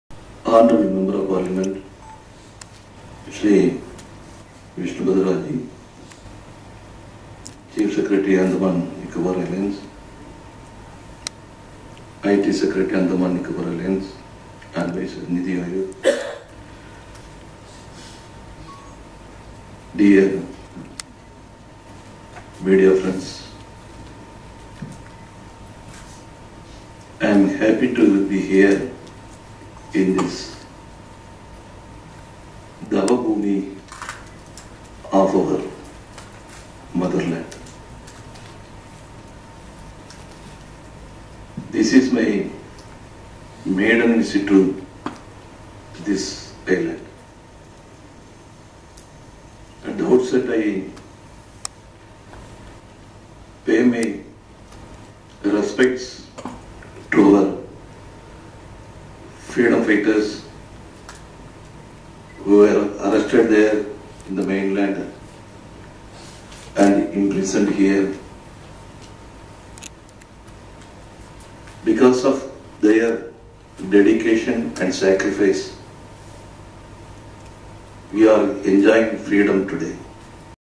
Hon'ble MinisterMoSPon RadhakrishnanPRESS CONFERENCE EdweepNews3
PRESS Conference by Hon’ble MoS Govt. Of India -(Road, Transport, Highways & Shipping)- Reg. Cashless Society –people friendly applications – Hon’ble Prime Minister rapid growth of implications – related his designated GoI updated applications – including present iTf - Digital Mela - in the presence of Hon'ble MP, Chief Secretary A&N Admn.,Special Secretary IT - Conference held at Megapode Nest Resort, Port Blair this evening – (MORE VISIT - VIDEO uploading) - thanking ALL !!!